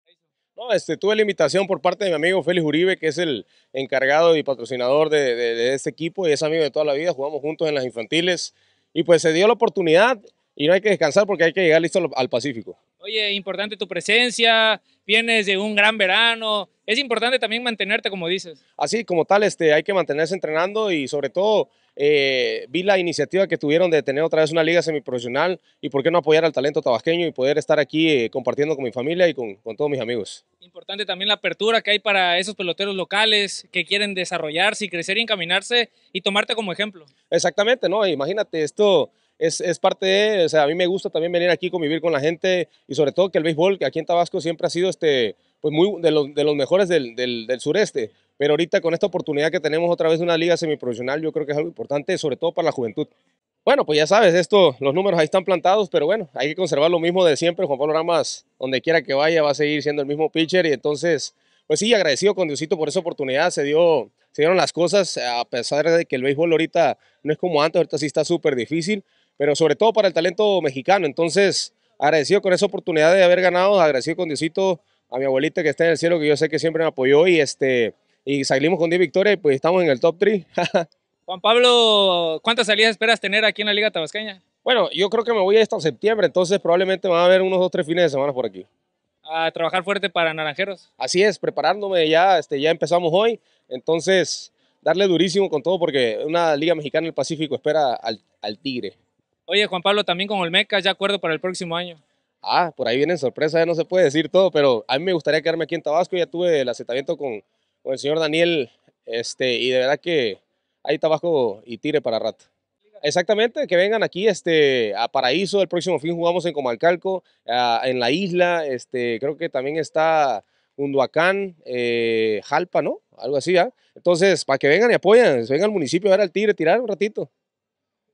Oramas🐅 lanzará con Cacaoteros🍫 para después tener un exitoso invierno❄️ con Naranjeros🍊
El líder de victorias de la LMB el Tigre de las Gaviotas Juan Pablo Oramas lanzará todo agosto con Cacaoteros de Comalcalco en la Liga Tabasqueña de Primera Fuerza, lo tomará como preparación para después tener un exitoso invierno con Naranjeros de Hermosillo en la Liga Mexicana del Pacifico